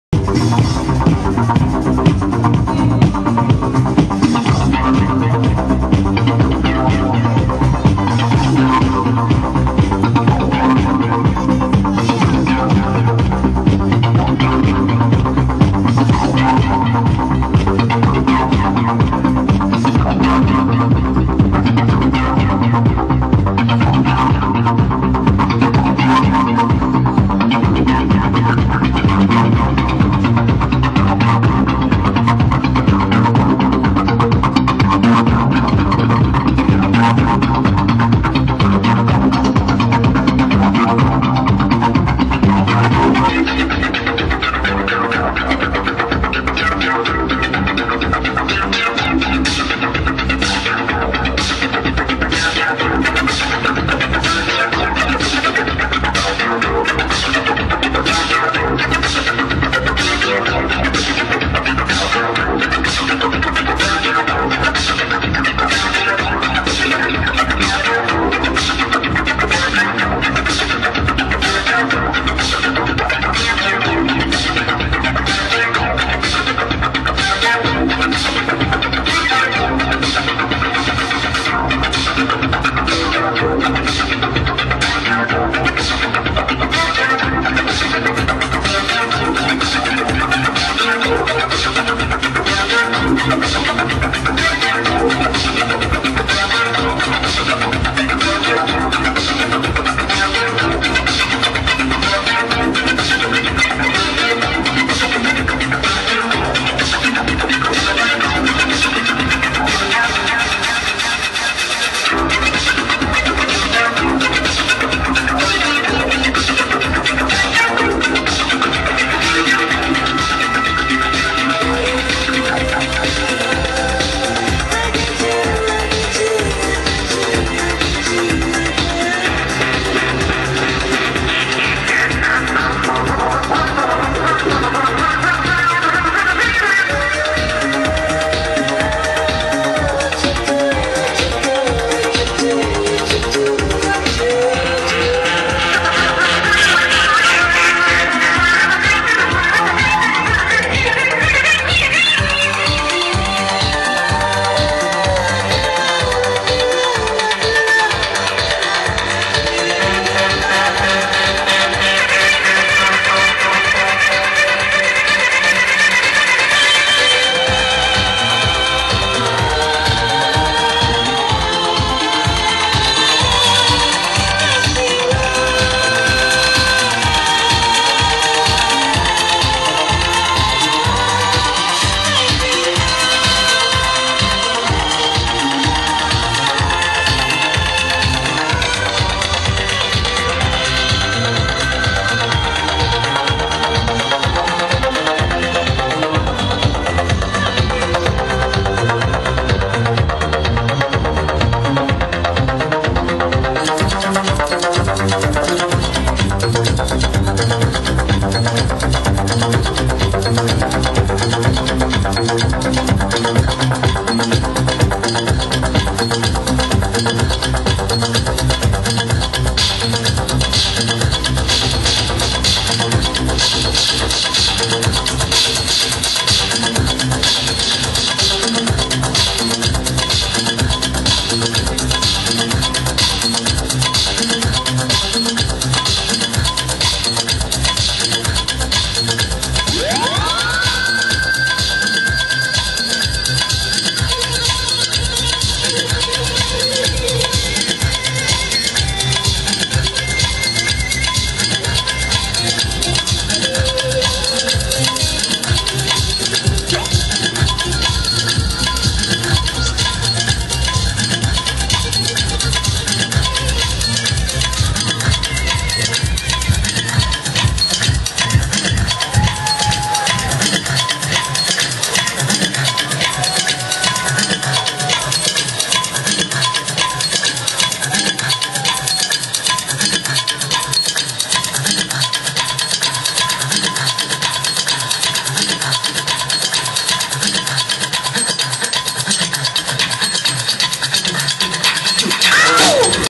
Deda Katzuma e Lugi live @Batti il tuo Tempo Festival, Bologna 8-6-2012
Deda aka Katzuma e Dj Lugi si esibiscono in via Filippo Re durante il Festival "Batti il tuo tempo" organizzato dal Collettivo Universitario Autonomo.